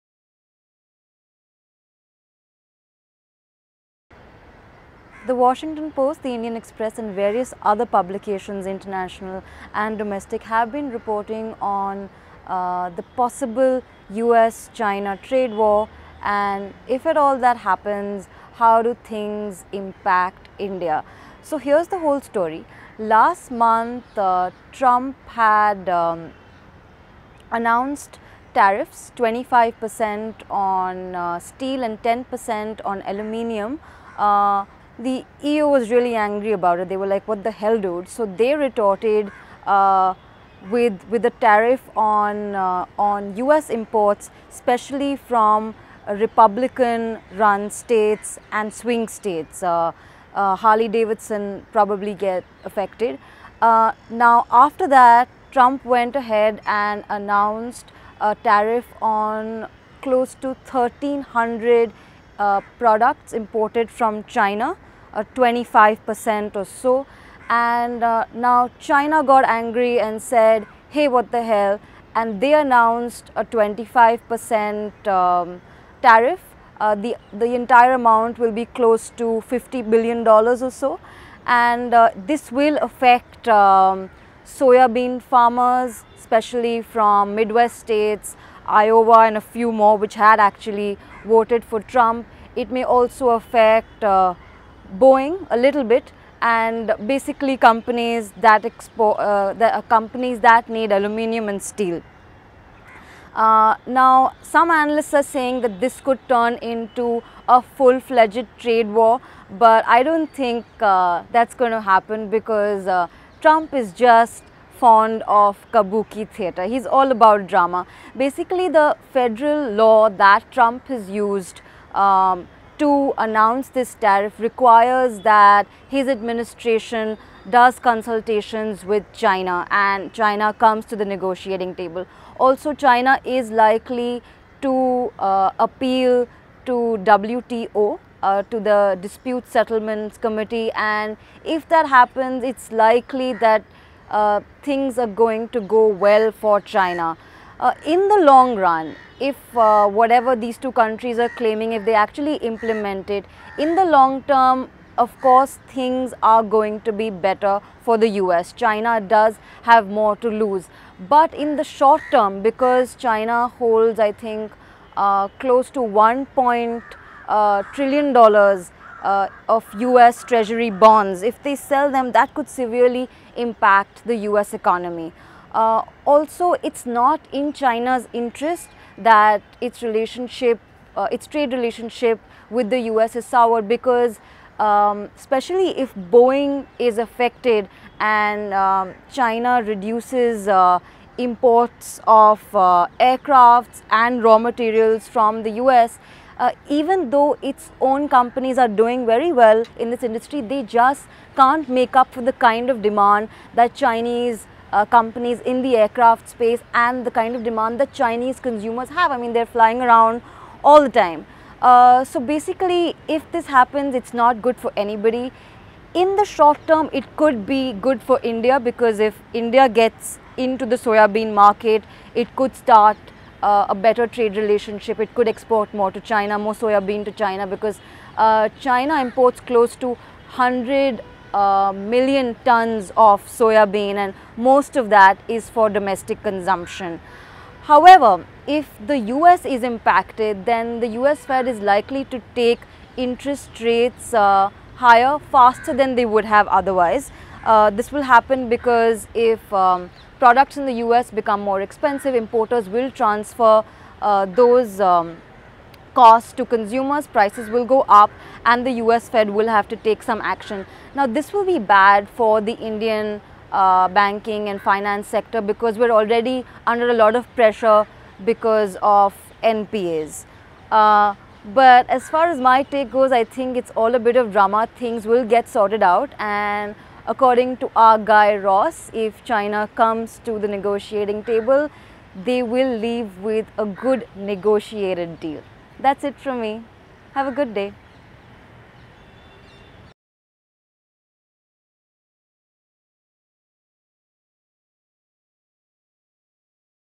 Trump Trade Tactics
Trump is on a tariff announcing spree. China is retaliating and analysts are afraid of a trade war. Boeing's stock dipped a bit and is recovering. Everybody is going nuts. Honestly, there's no reason to coz China is going to come to the negotiating table and leave with a "negotiated deal".

India may stand to gain from the US-China trade wa